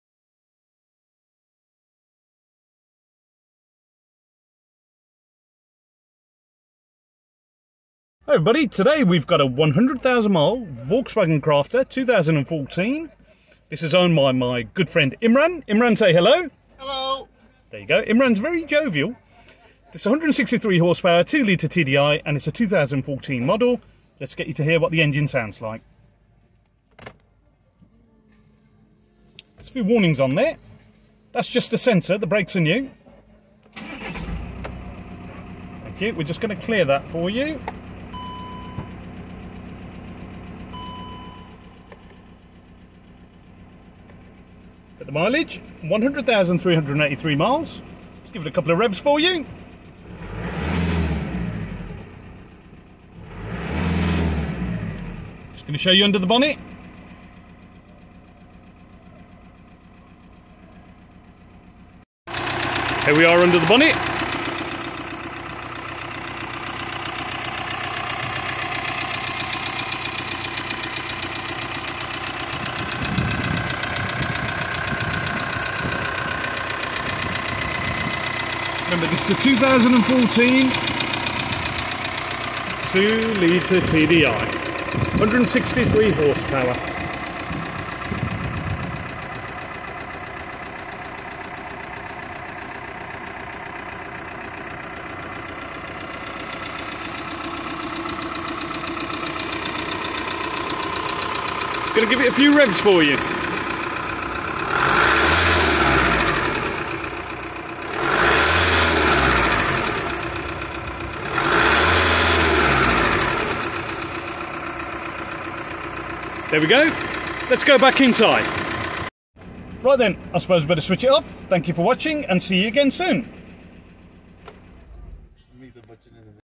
Engine Sounds 2014 VW Crafter 2.0 TDI 163 Bhp 100k Miles Engine Start Up and Sound
Many thanks to my good friend Imran for the use of his van for filming this video